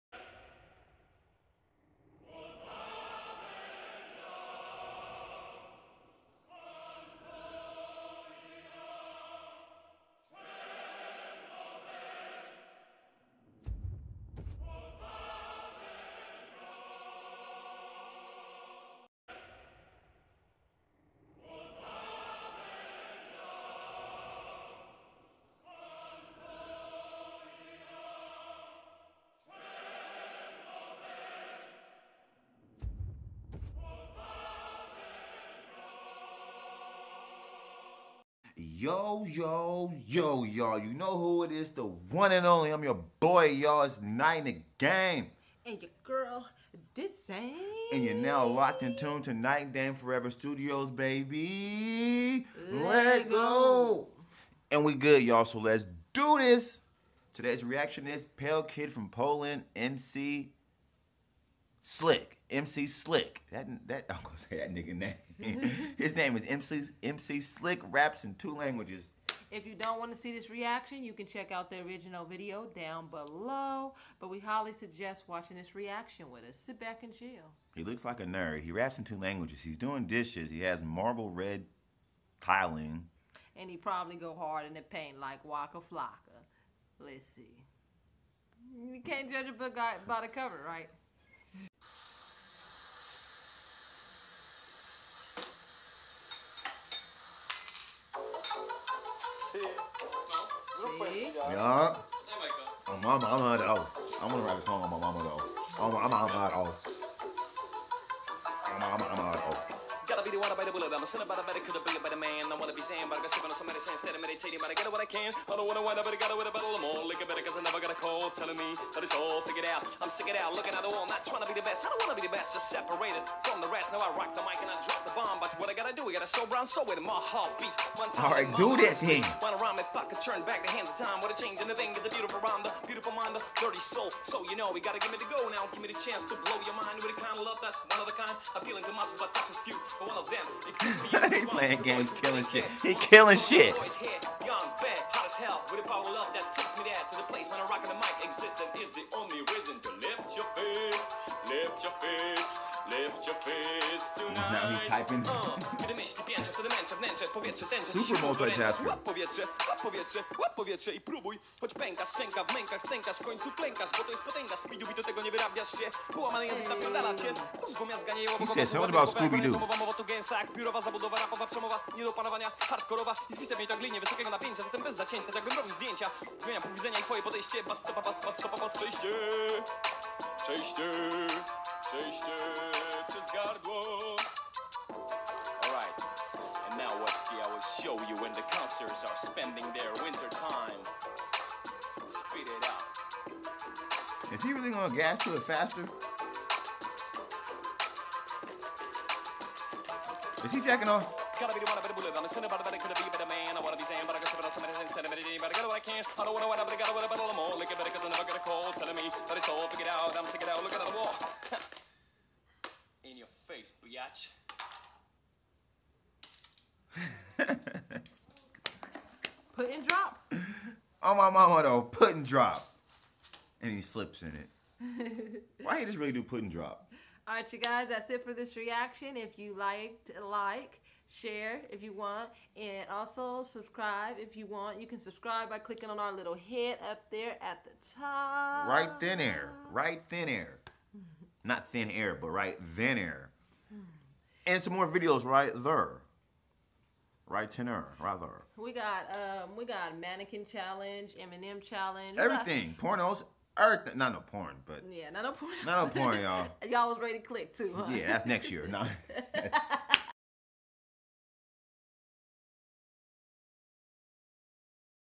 Pale kid from Poland - MC 'Silk', raps in 2 languages (SILKSPACE) -- K&D Reacts
SMASH that like button League & WATCHERS!

Twitter: Ace & Naj The Family Gr¡nd

So League, tell us what's Next?

Lei Goooo!